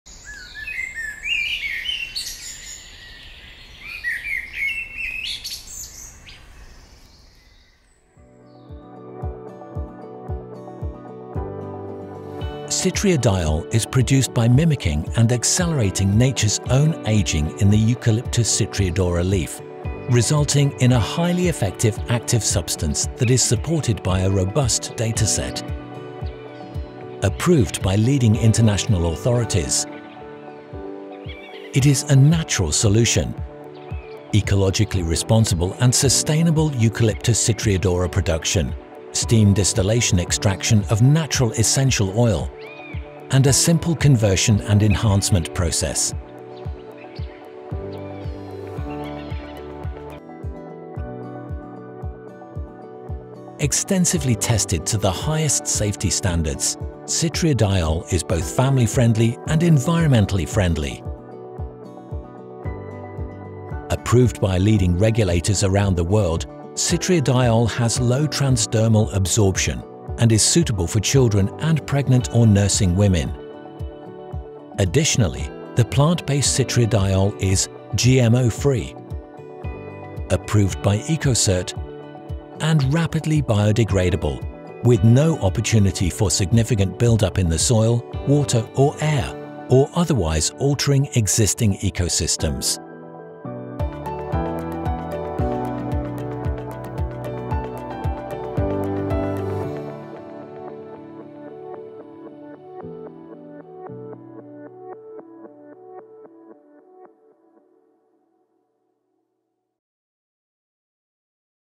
Citriodiol - Nature’s Insect Repellent
A natural, plant-based insect repellent safe for the whole family and the environment with scientifically proven effectiveness